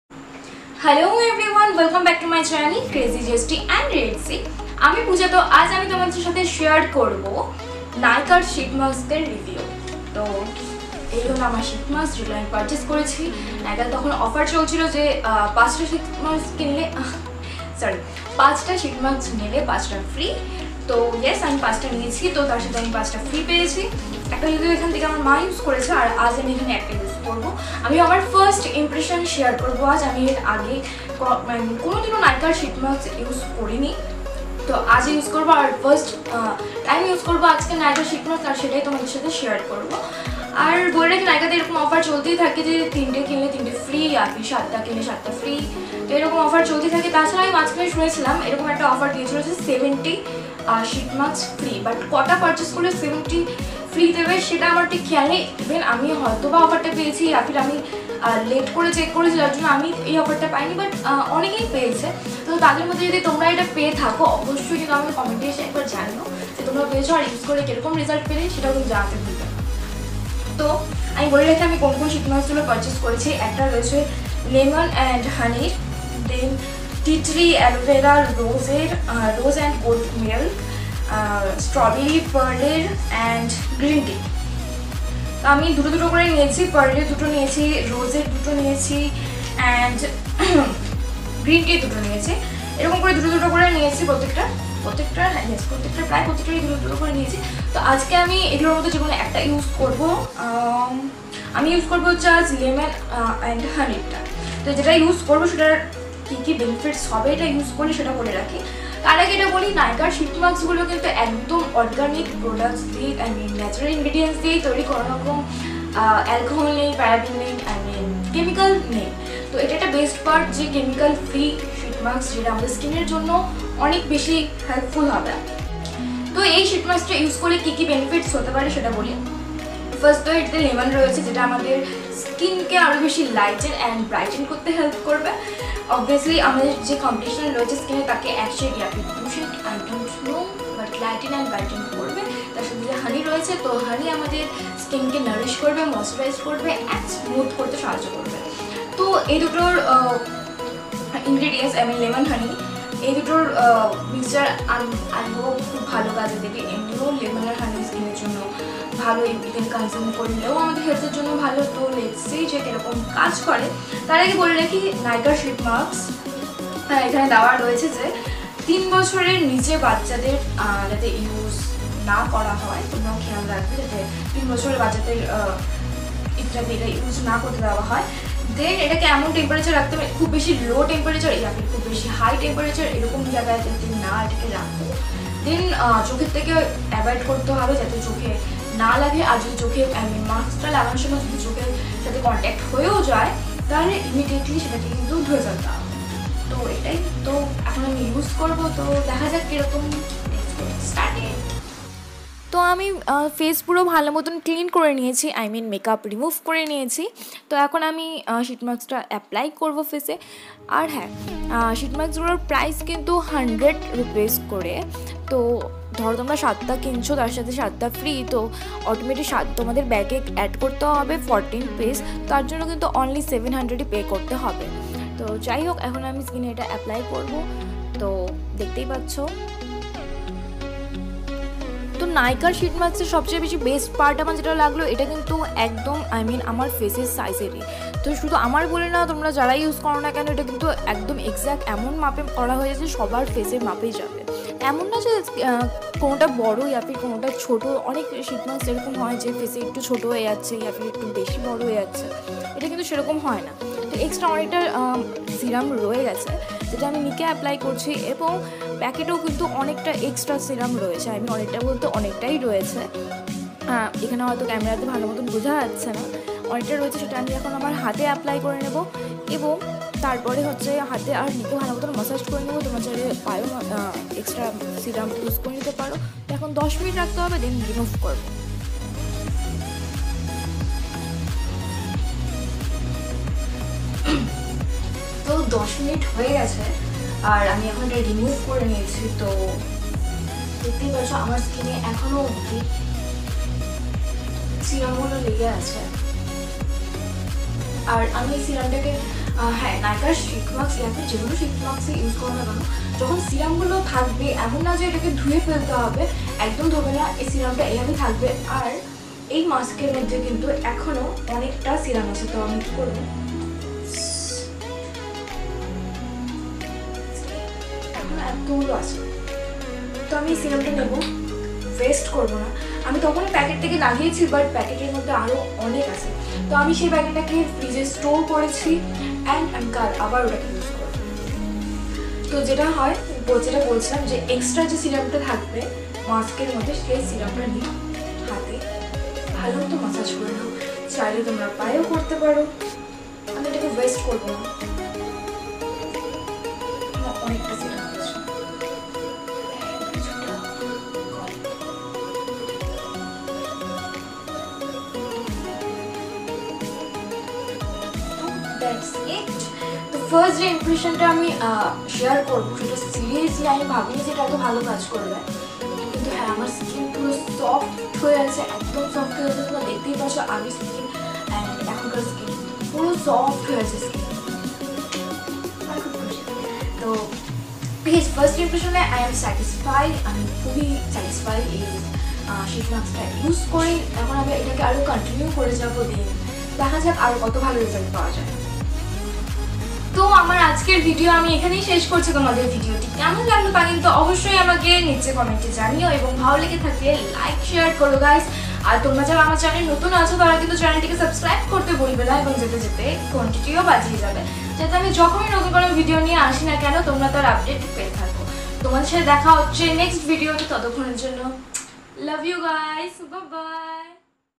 NykaaSheetMask Nykaa Sheet Masks First Impression, Review & Demo|Crazy Zesty Ritzy|Puja Roy
Hey there!

Glowing Brightens lightens Skin

Meet me on Tiktok - @Pujaroy4

Xoxo ❤
Puja